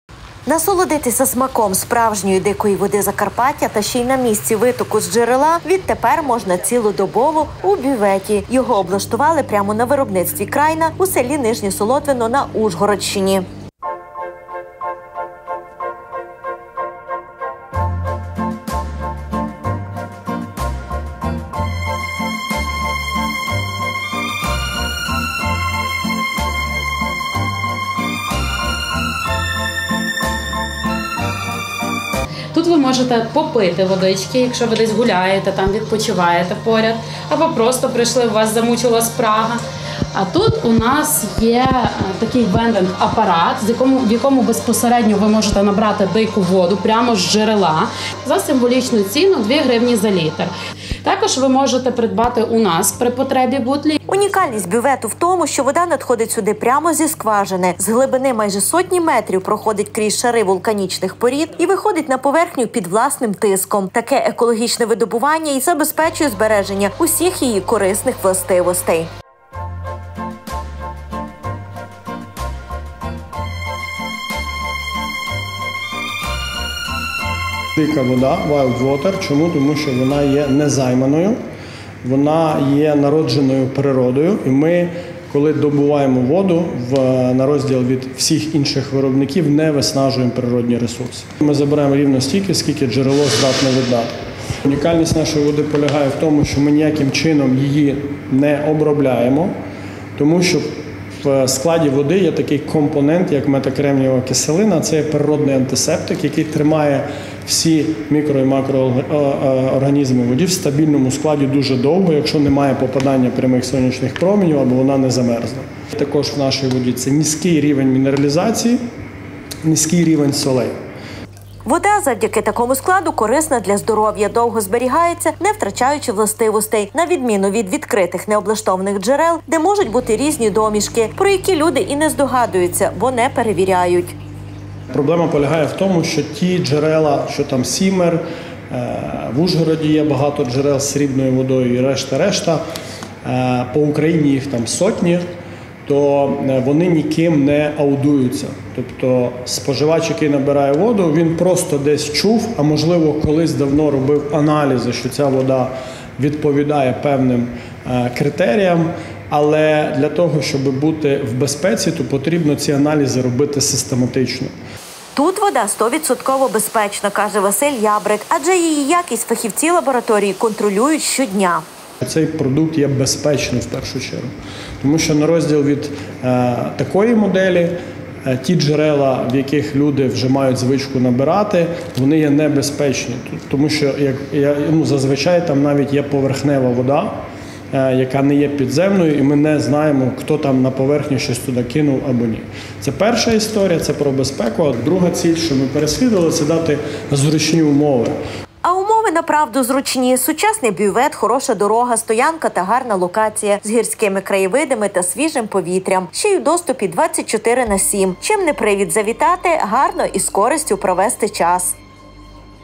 Бювет Крайна: природне джерело здоров'я
Бювет з природньою, так званою, дикою водою відкрили на виробництві Крайна. Це у селі Нижнє солотвино, 55А на Ужгородщині. Відтепер цілодобово якісна вода доступна для всіх бажаючих у сучасному бюветі з облаштованою територією поруч і у гарній гірській локації. Насолодитися смаком справжньої дикої води Закарпаття та ще й на місці витоку з джерела відтепер можна цілодобово у бюветі. Його облаштували прямо на виробництві Крайна у селі Нижнє Солотвино на Ужгородщині. Унікальність бювету в тому, що вода надходить сюди прямо зі скважини. З глибини майже сотні метрів проходить крізь шари вулканічних порід і виходить на поверхню під власним тиском. Таке екологічне видобування й забезпечує збереження усіх її корисних властивостей. Вода завдяки такому складу корисна для здоров’я, довго зберігається, не втрачаючи  властивостей. На відміну від відкритих необлаштованих джерел, де можуть бути різні домішки. Про які люди і не здогадуються, бо не  перевіряють. Тут вода стовідсотково безпечна, адже її якість фахівці лабораторії контролюють щодня! А умови направду зручні: сучасний бювет, хороша дорога, стоянка та гарна локація з гірськими краєвидами та свіжим повітрям. Ще й у доступі 24 на 7. Чим не привід завітати, гарно і з користю провести час?